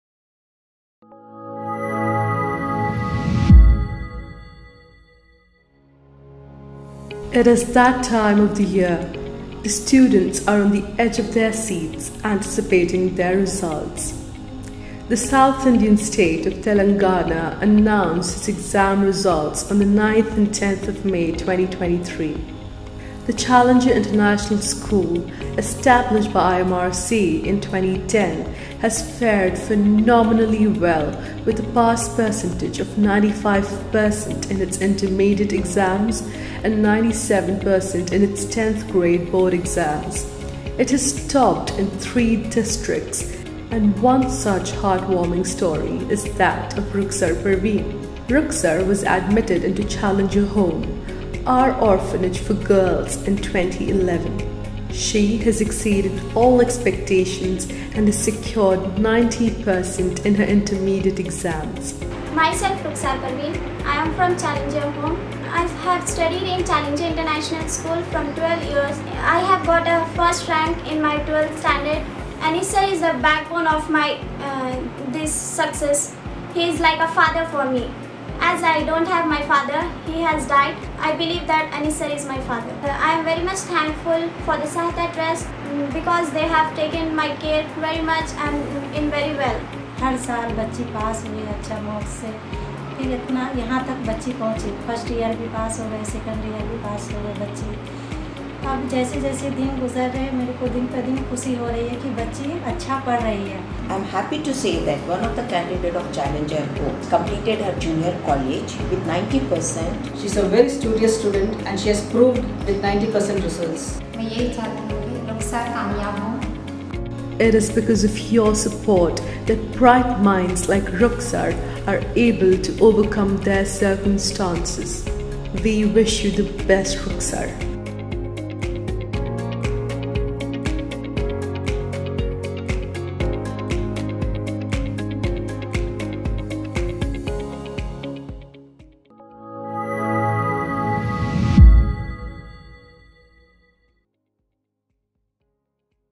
The Success Story of Ruksar Parveen
The Success Story of Ruksar Parveen. She started with IMRC at the Challenger Home, a girls home for orphan and destitute children, and was a enrolled at the Challenger International School. Through support from our donors towards our Educational Program, it's students like Ruksar who could study from elementary school to junior college at a competitive institution with full support. Today, Ruksar has secured 90% of her intermediate exams and has dreams for a career in science. Jazak Allah khair for your support!